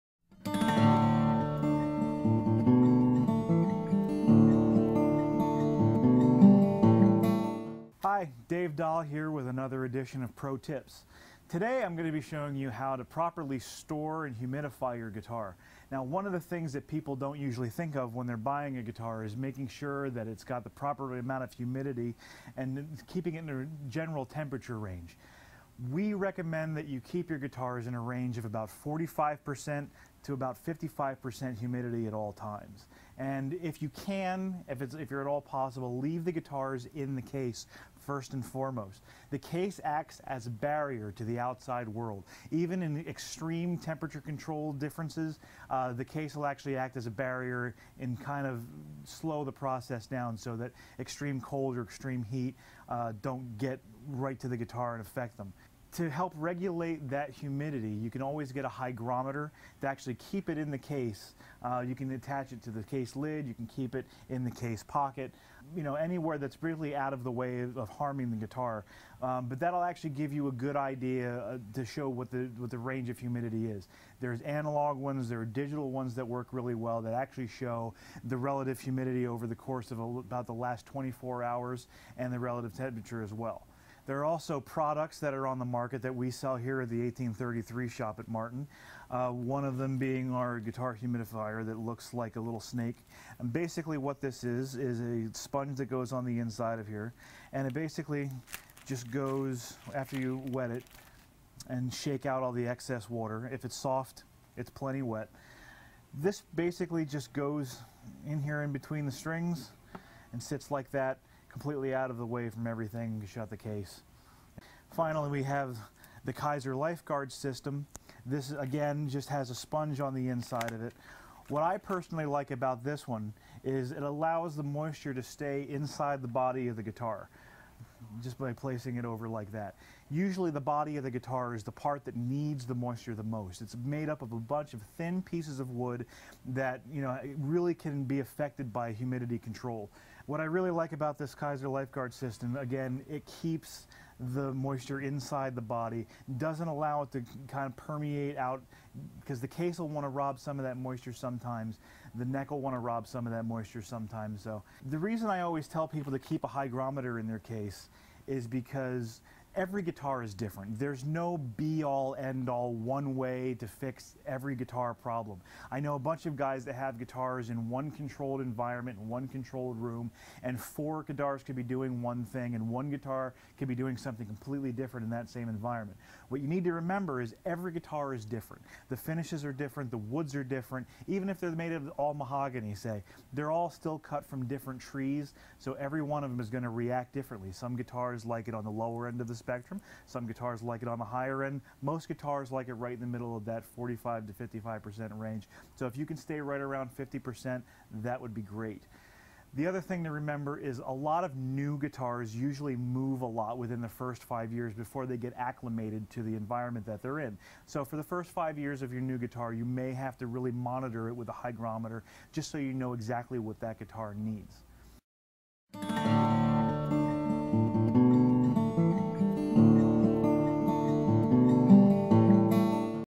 How to Protect your Acoustic Guitar from Humidity
Don't let humidity get the best of your guitar! Learn these tips for keeping it safe thanks to the pros. Also view all of the Martin Guitars we craft